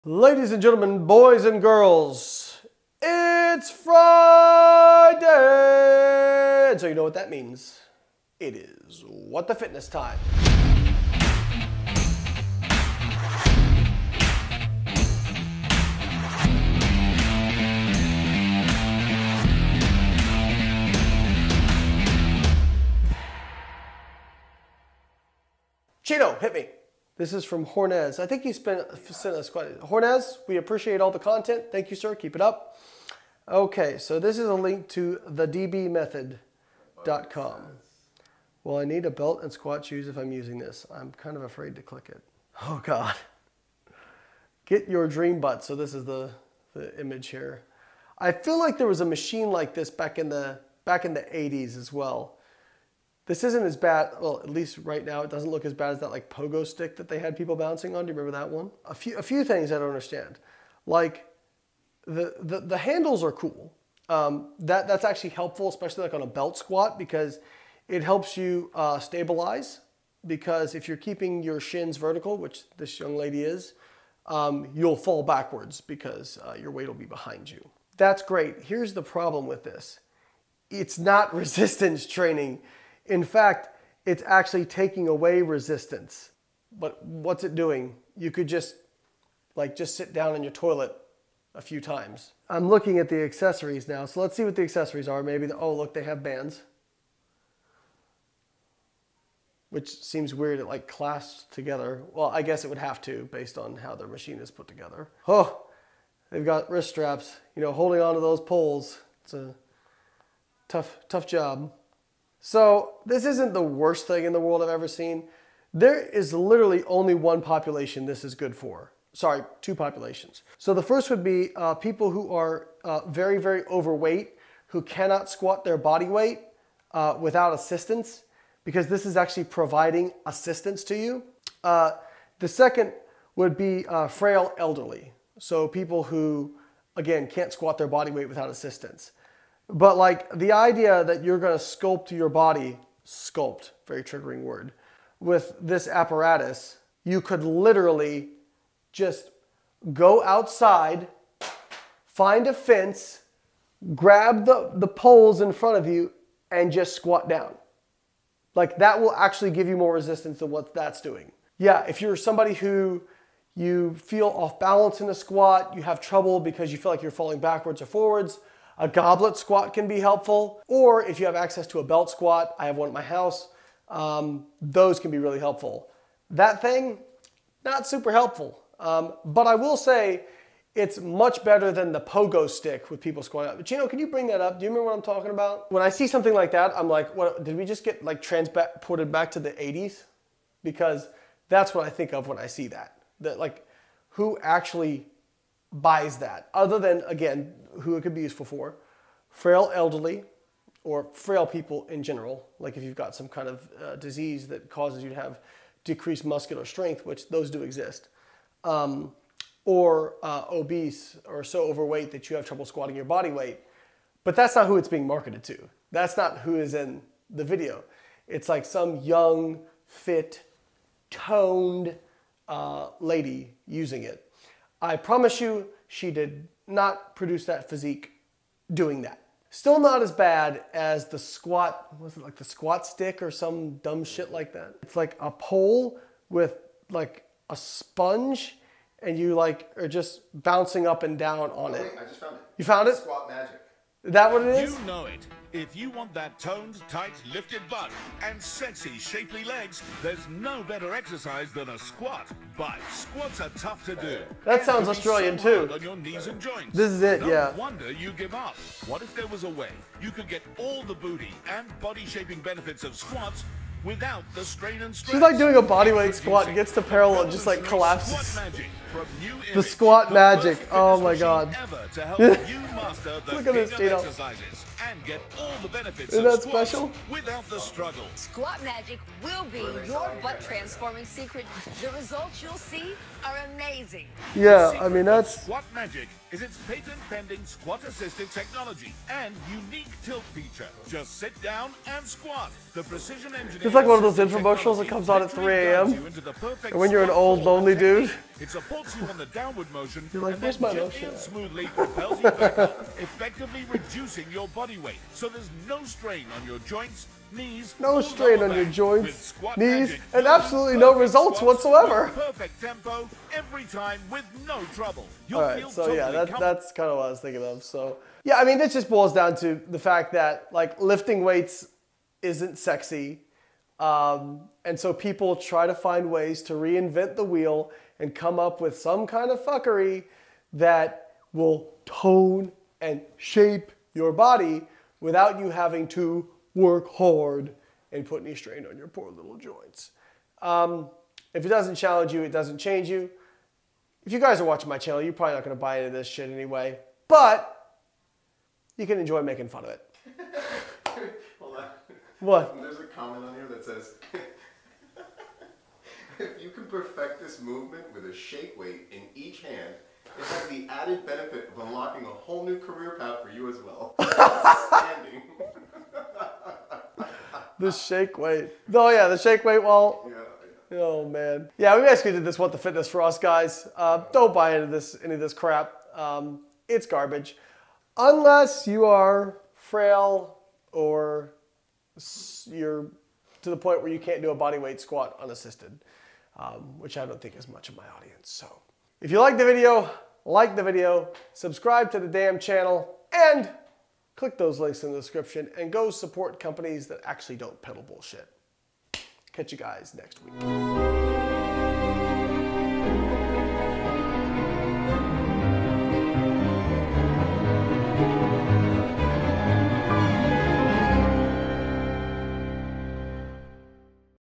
Squats for the modern man & woman... What The Fitness
Well, I can say with certainty this isn’t the worse thing I’ve ever seen. But that still doesn’t make it a good product. Let’s talk squats, it’s what the fitness time!